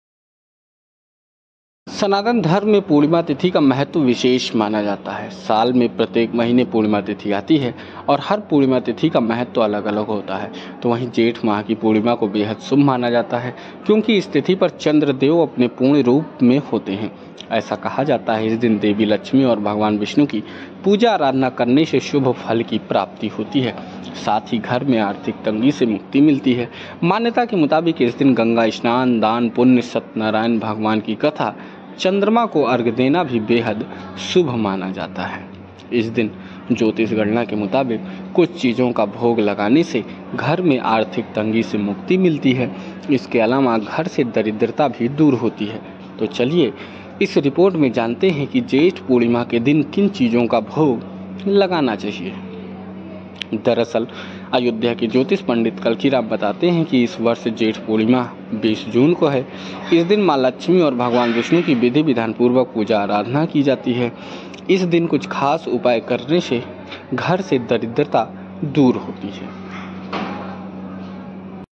Jyeshtha Purnima 2024: ज्येष्ठ पूर्णिमा पर करें ये कारगर उपाय, घर आएगी सुख-समृद्धि!
Ayodhya's astrologer Pandit Kalki Ram says that this year the full moon of Jyeshtha month is on 22 June, so on this day there is a law to worship Goddess Lakshmi and Lord Vishnu. Offering certain things on this day removes poverty from the house.
अयोध्या के ज्योतिषी पंडित कल्कि राम बताते हैं कि इस वर्ष  ज्येष्ठ  माह की पूर्णिमा 22 जून को है, तो इस दिन मां लक्ष्मी और भगवान विष्णु की पूजा आराधना करने का विधान है. इस दिन कुछ खास चीजों का भोग लगाने से घर से दरिद्रता दूर होती है.